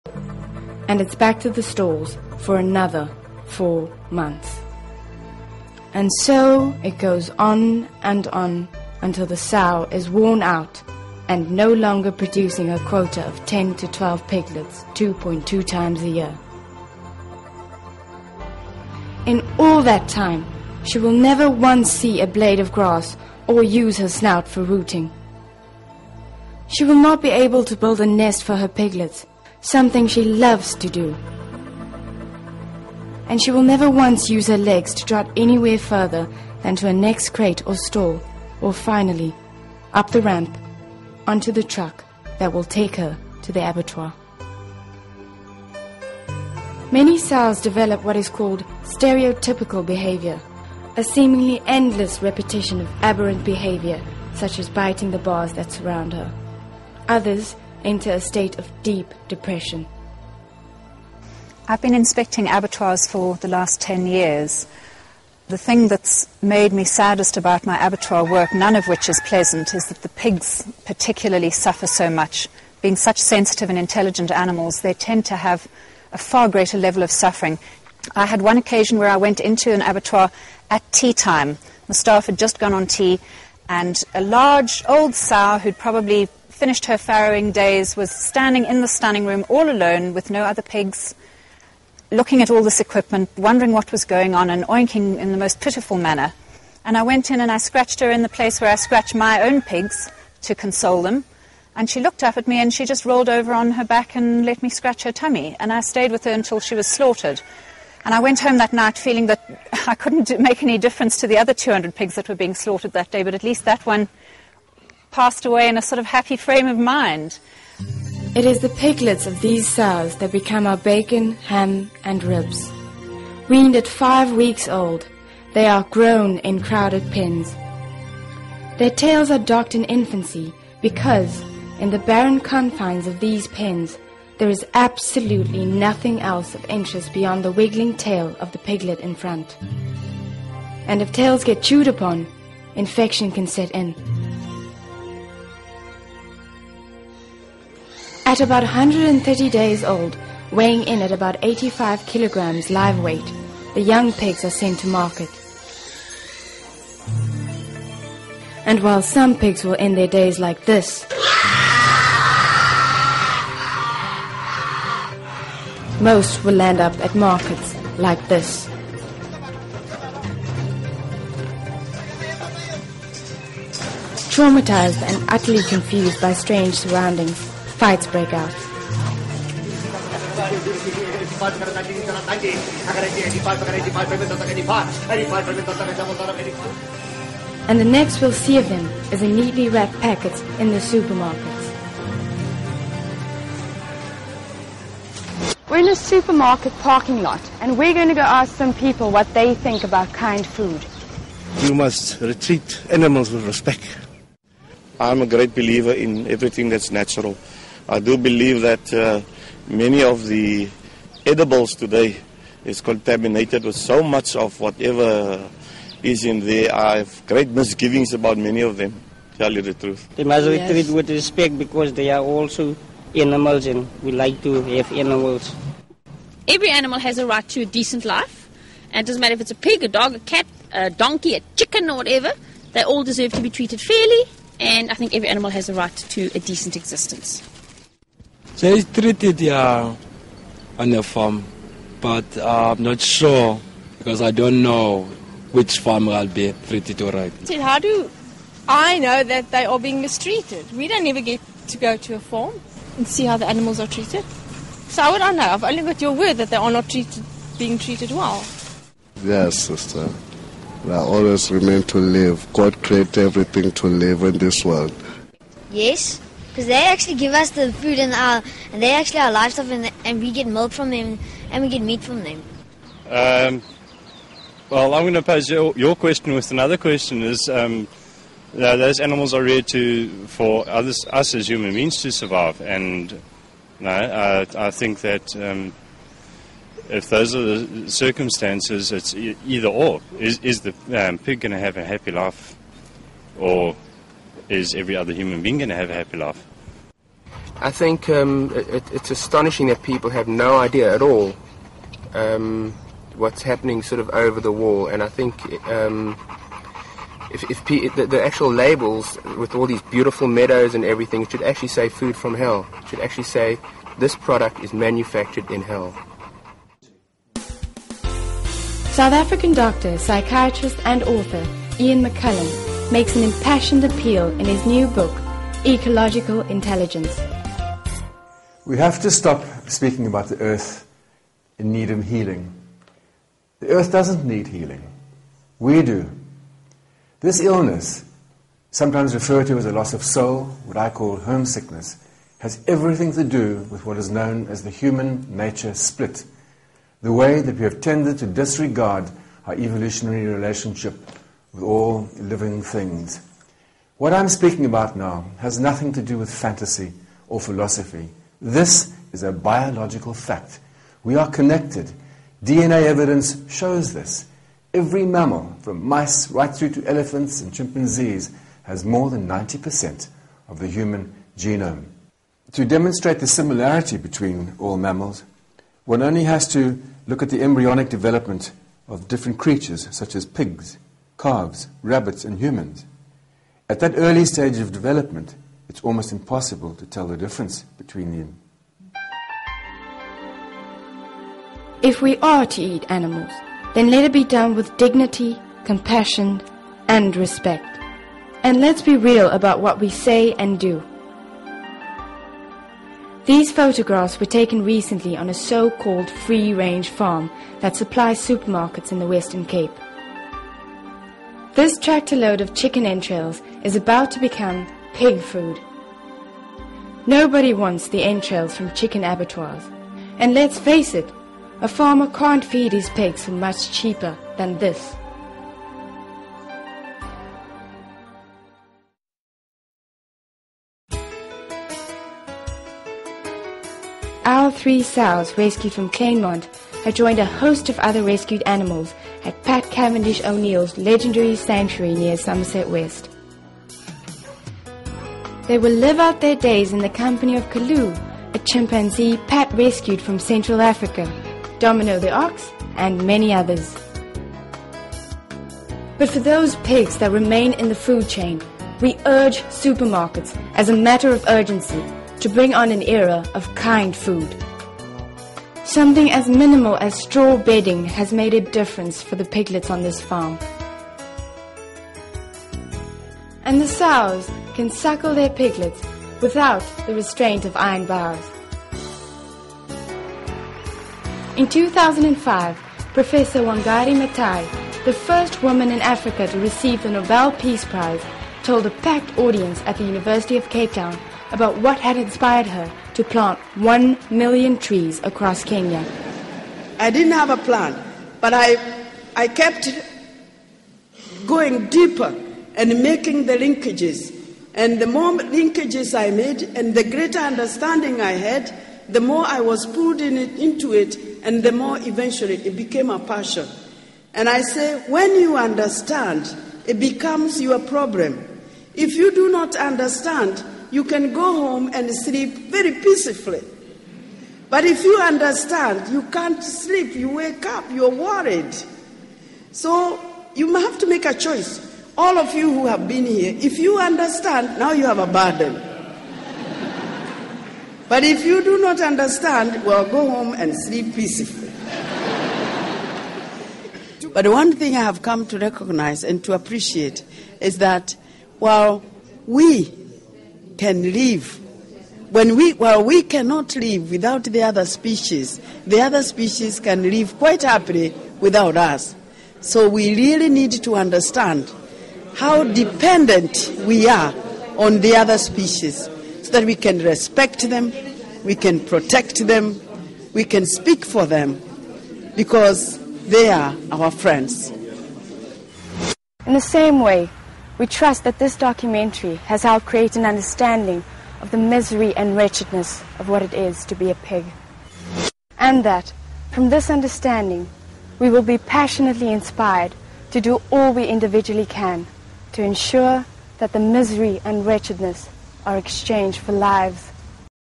In Search of Kind Food - Spotlight on Pigs (Part 2 of 2)
Born to mothers behind bars, raised in squalor and deprivation, slaughtered as juveniles... That's our bacon - A South African Perspective.